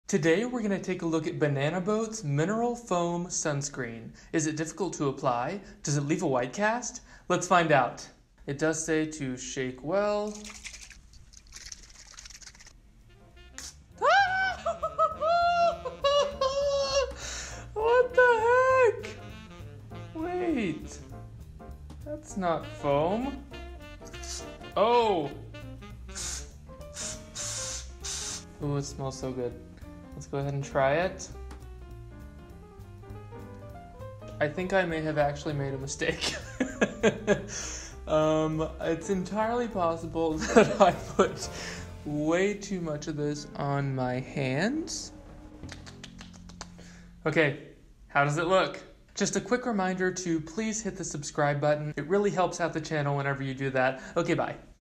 trying Banana Boat's mineral foam sunscreen
Is it difficult to apply? Does it leave a white cast? Let's find out!

Today, we take a look at Banana Boat's Kids SPF 50 Mineral Foam Sunscreen with vanishing blue foam. This sunscreen has a "berry blast" scent.

I bought this with the Banana Boat sunscreen that I reviewed the other day. When I saw this on the shelf I just had to try it. I mean come on... a sunscreen foam??? The bottle also said that the blue foam would become clear when applied so I was excited to put that to the test.

This channel is intended to be a fun place to learn about sunscreen and other sun care products.

***sun care advocate, NOT a medical professional***